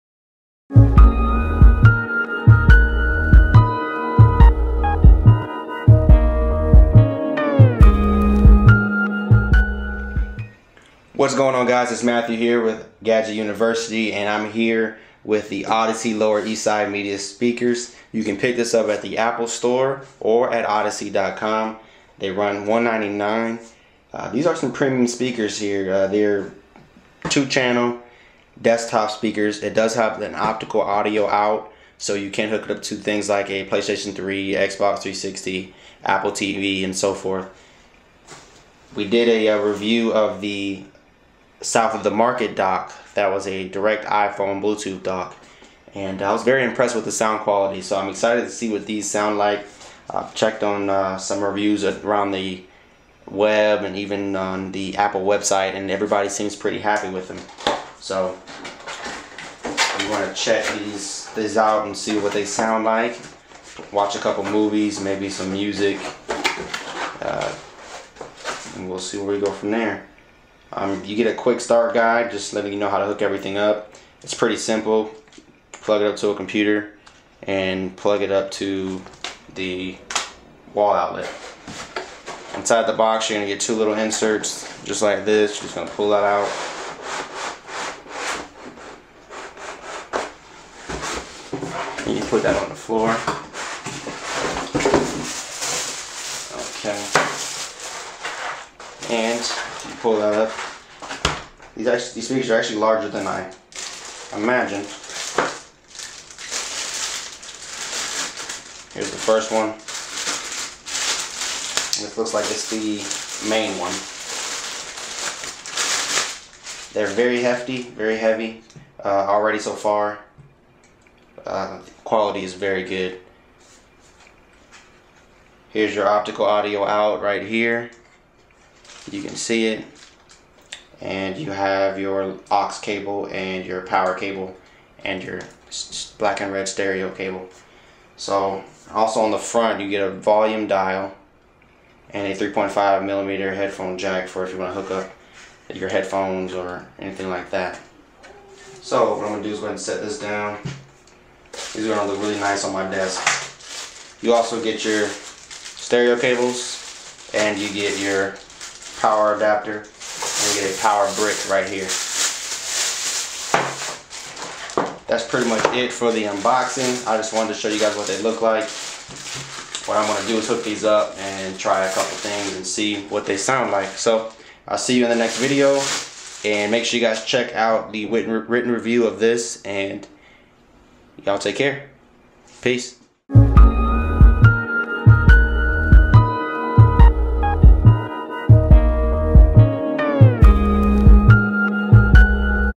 Audyssey Lower East Side Media Speakers Unboxing
Coming in at a price of $199, you have to wonder, can Audyssey really compete at that price. To be honest, I think so. They took a risk with their aggressive pricing and obviously it has paid off. Apple doesn't just sell any audio equipment. They pick the creme of the crop. Check out a local Apple store and try these out. I assure you. An Apple employee will probably tell you to turn these babies back down.